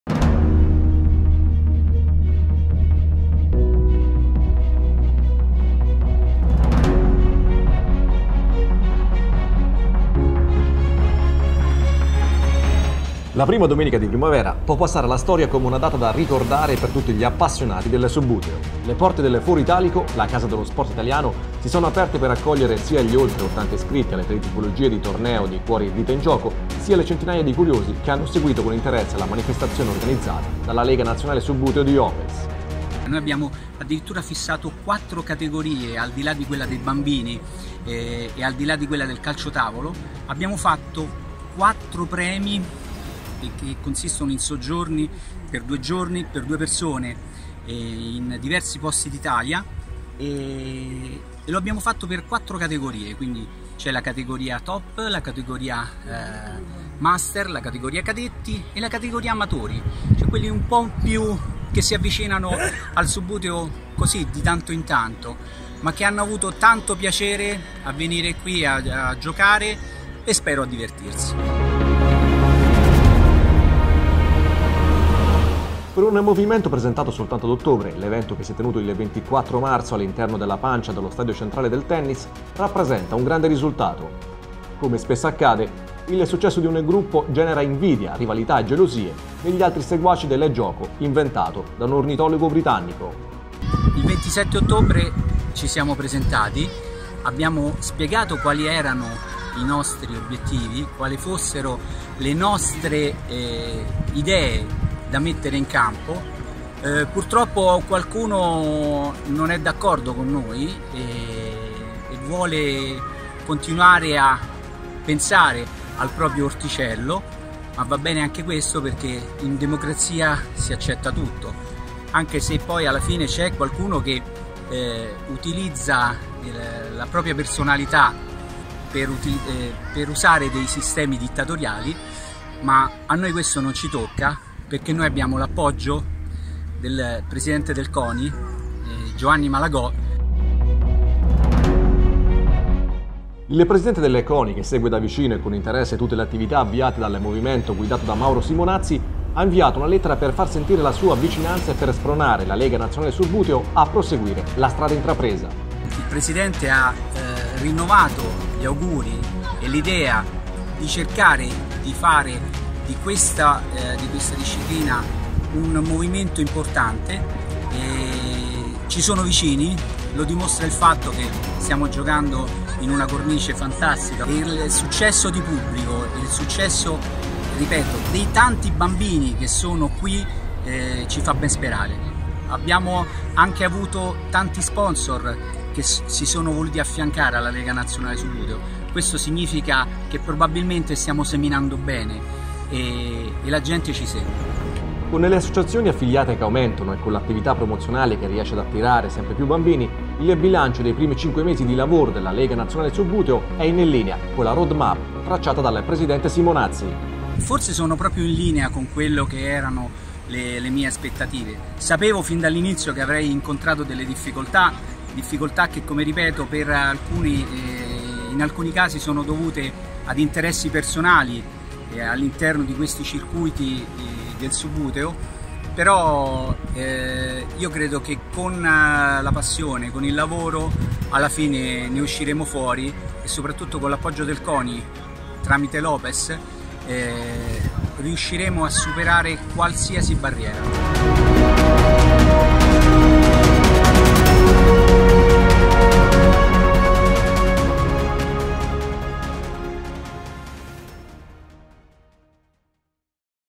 Intervista al Foro Italico
Il Presidente LNS parla del torneo e non solo